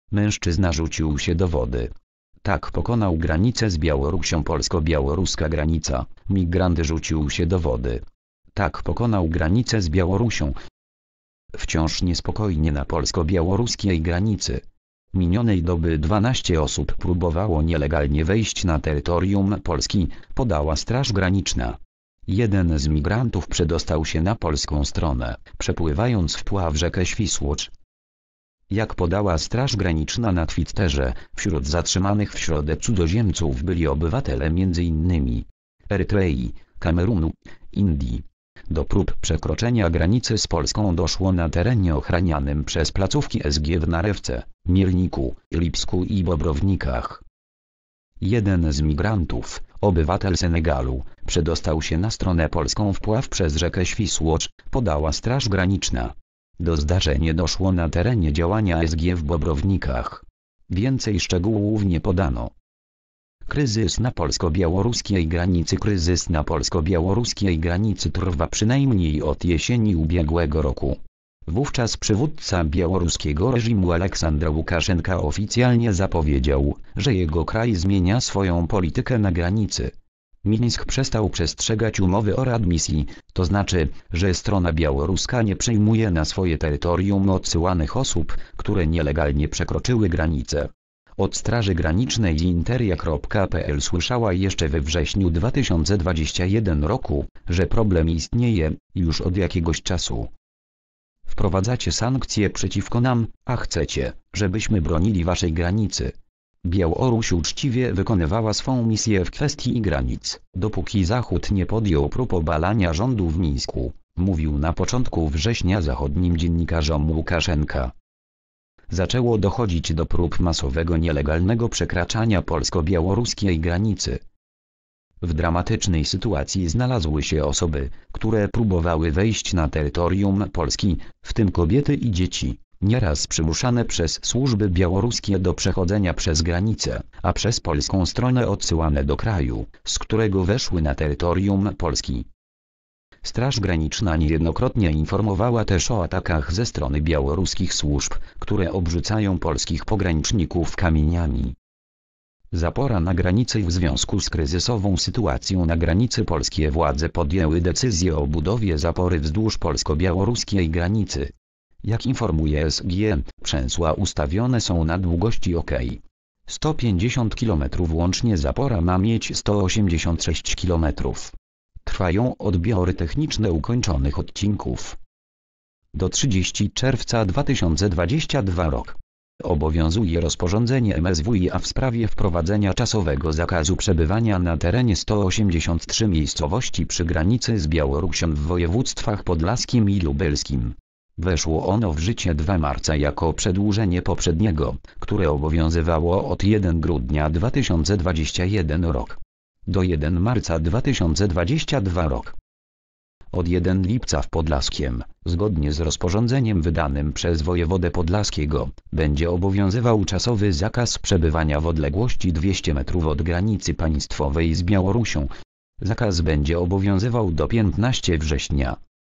Mężczyzna rzucił się do wody. Tak pokonał granicę z Białorusią.
Polsko-białoruska granica: Migrant rzucił się do wody. Tak pokonał granicę z Białorusią.

  (fot. Straż Graniczna)